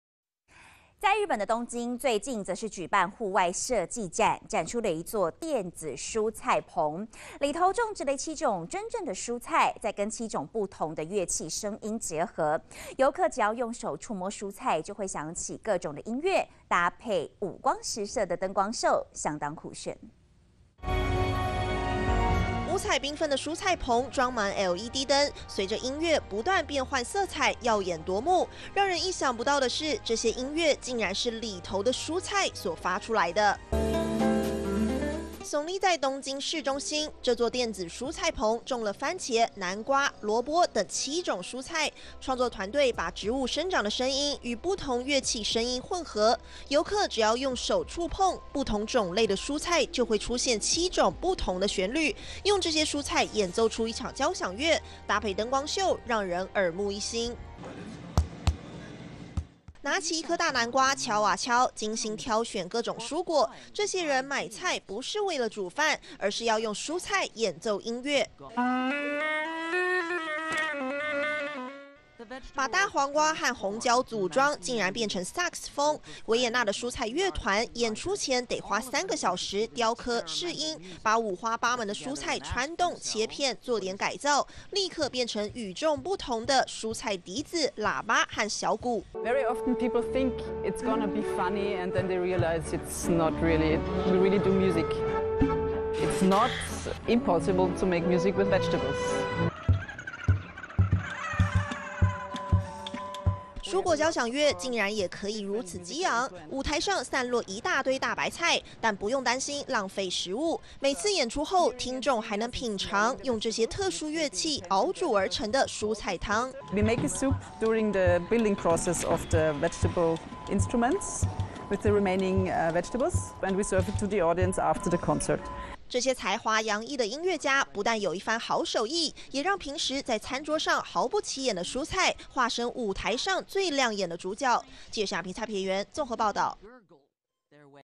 電子蔬菜棚 用蔬菜來表演一場酷炫的交響樂燈光秀
夢幻蔬菜棚！用蔬菜來表演一場酷炫的交響樂燈光秀吧 電子蔬菜棚觸控奏出交響樂 你想過用蔬菜演奏一場交響樂，順便再來一場酷炫的燈光秀嗎？最近在日本，一個創意團隊「PARTY」在東京市中心建了一座特別的「電子蔬菜棚」。當你觸摸不同種類的蔬菜，就能聽到不同樂器的聲音，配合棚內絢麗多彩的LED燈光變化，給人一種迷人的「溫室效應」。

「科技新報」報導，這套創意設備名為「電子蔬菜」（Digital Vegetables），但這裡的蔬菜都是種在土壤裡的「真蔬菜」。棚內種了七種蔬菜，團隊的聲音設計師把植物生長的聲音記錄下來，再用電腦與不同樂器聲音混合，合成七種不同的旋律。當你用手觸碰到這些蔬菜時，「樂器」就發出不同的音聲。

其中番茄是小提琴、紅蘿蔔是小號、蘿蔔是長笛、高麗菜是雙簧管、南瓜是單簧管、番薯是鋼琴、茄子是豎琴。

其實在奧地利也有一個「維也納蔬菜樂團」，他們將蔬菜改造成樂器來演奏交響樂。而「電子蔬菜棚」的創意毫不遜色，還能讓更多人近距離體驗。

除了讓蔬菜變身「樂器」，棚內絢麗的燈光也會隨之變化。到了晚上，棚架每隔十五分鐘就會來一場燈光秀，七色燈不斷變換色彩。其實這些燈光的變化也與植物有密切關係，顏色反映了七種蔬菜從種子到葉子、花朵和果實的生成過程中不同階段的顏色，比如當民眾觸摸蘿蔔根部時，就會看到棚頂發出細長的白光。

每種蔬菜旁還有詳細的介紹，讓參觀者了解蔬菜的生長過程。展覽負責人伊藤直樹表示，「很多城市裡的孩子，沒見過長在土裡的蔬菜！」他希望讓更多人享受展覽的同時，獲得關於蔬菜的知識。

「電子蔬菜棚」是日本今年Tokyo Midtown DESIGN TOUCH展覽的一部分，這個活動從二○○七年開始舉辦，主推「讓人們用五感感受未來」的設計，每年都會集結國內外優秀設計師，讓市民近距離體驗不同的生活設計。